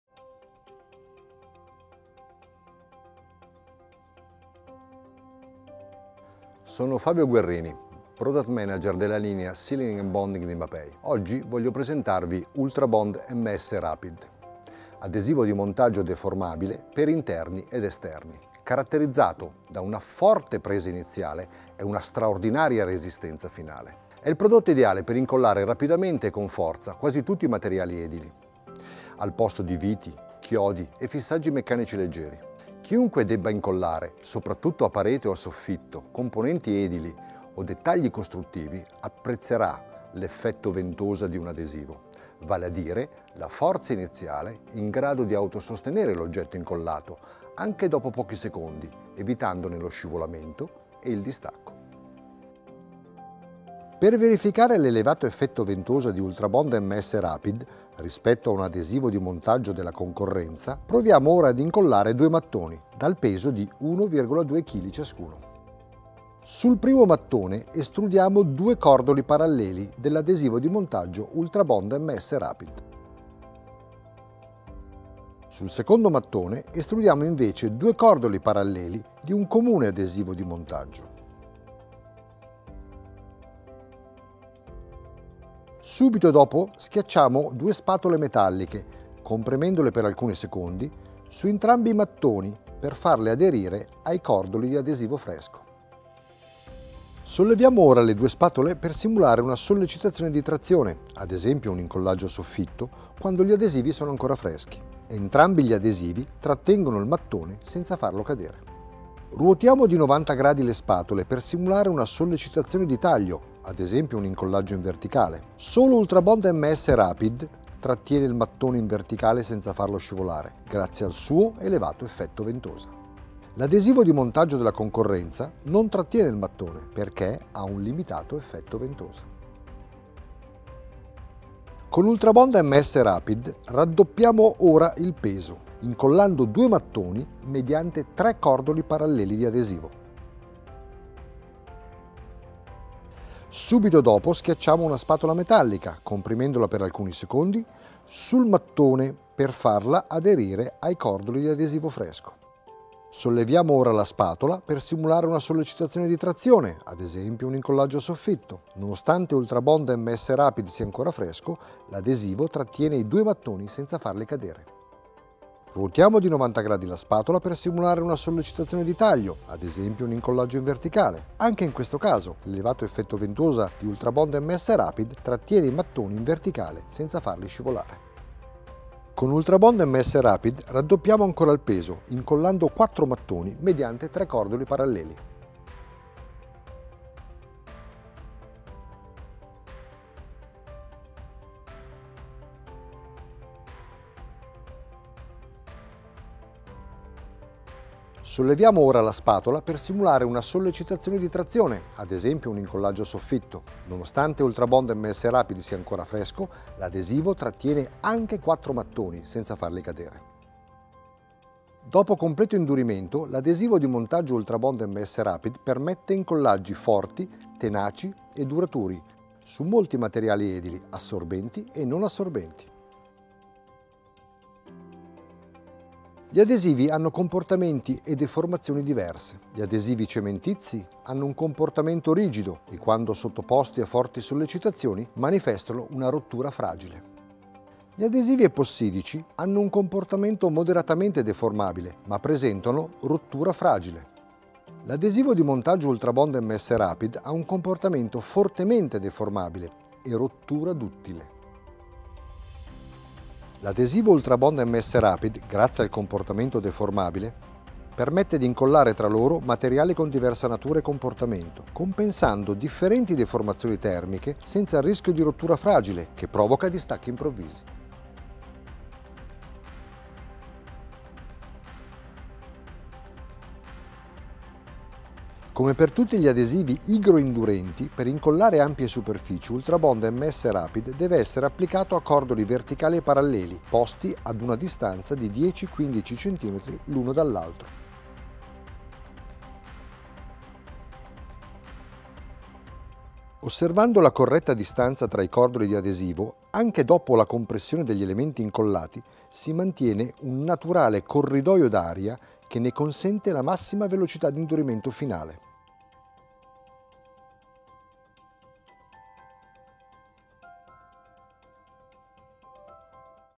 Adesivo di Montaggio Rapido | Deformabile | Per interni ed esterni | Ultrabond MS Rapid
ULTRABOND MS RAPID è l'adesivo di montaggio specifico per eseguire incollaggi deformabili su quasi tutti i materiali edili, anche umidi.
Riduce o elimina l’uso di fissaggi meccanici.
Ideale per incollare in interno ed esterno, elementi costruttivi leggeri e pesanti.